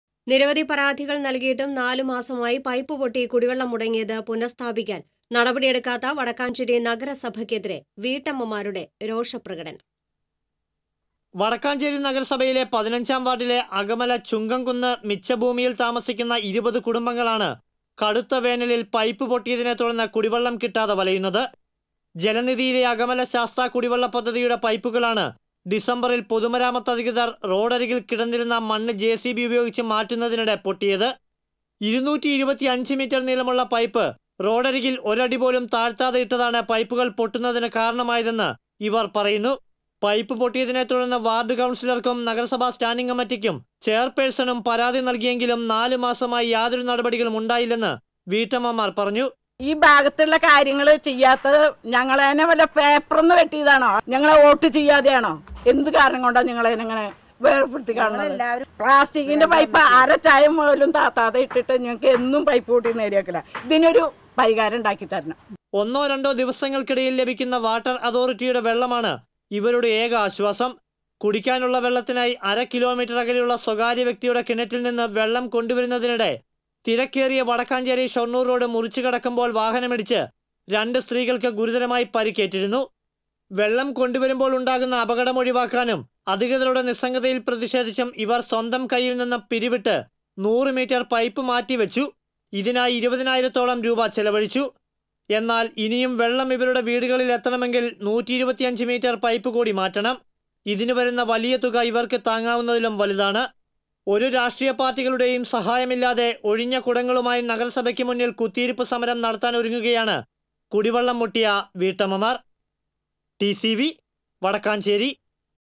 കുടിവെള്ളം പുനഃസ്ഥാപിക്കാത്തതിൽ പ്രതിഷേധം | T.C.V. Wadakkanchery
നിരവധി പരാതികള്‍ നല്‍കിയിട്ടും, നാലു മാസമായി പൈപ്പുപൊട്ടി കുടിവെള്ളം മുടങ്ങിയത് പുനഃസ്ഥാപിക്കാന്‍ നടപടി എടുക്കാത്ത വടക്കാഞ്ചേരി നഗരസഭക്കെതിരെ വീട്ടമ്മമാരുടെ രോഷപ്രകടനം.

Camera & Report: Arun Das